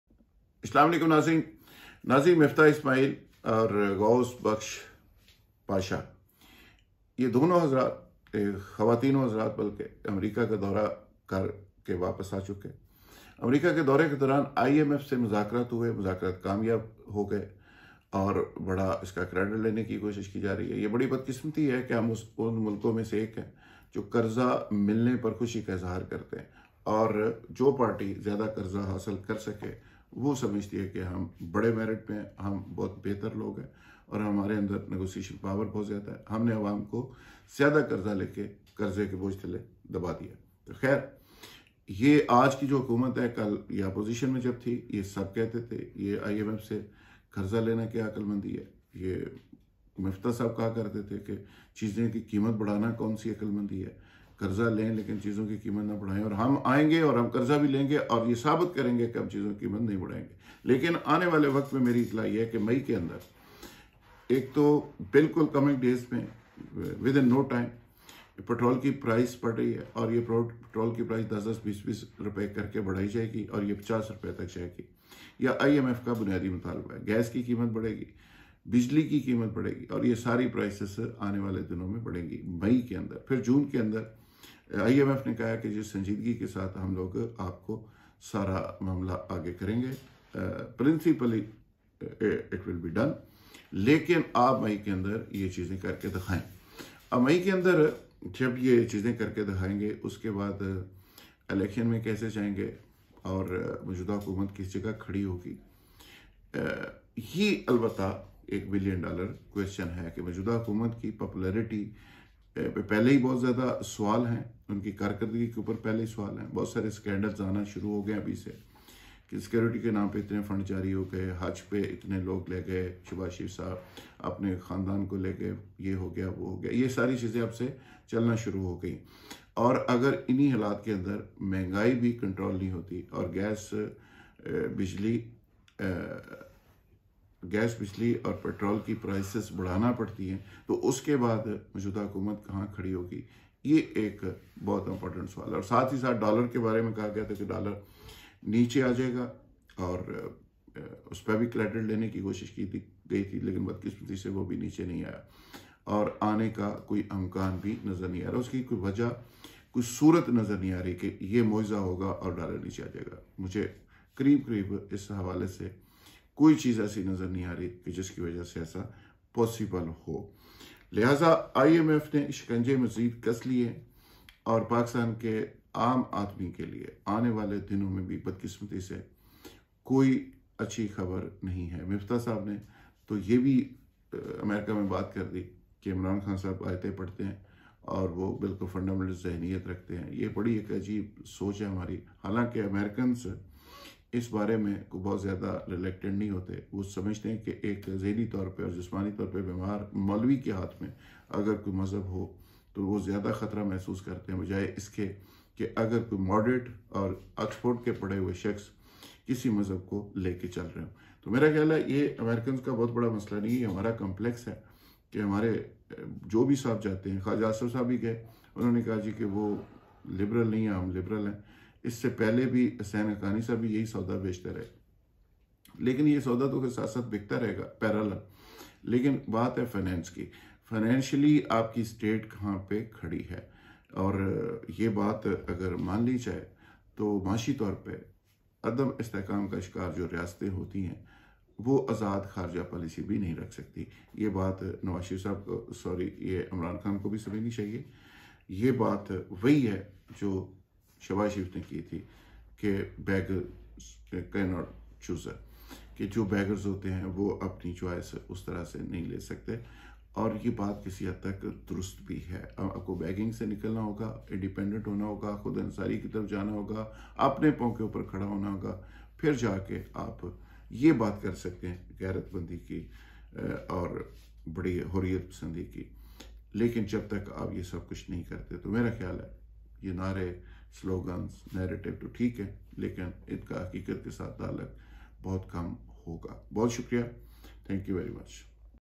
26 April 2022
پٹرول ۔بجلی اور گیس مہنگی۔فیصلہ ہوگیا

 آئی ایم ایف نے کڑی شرائط رکھ دیں

عوام کو بجلی گیس اور پٹرول کی مہنگائی جلد ملیں گے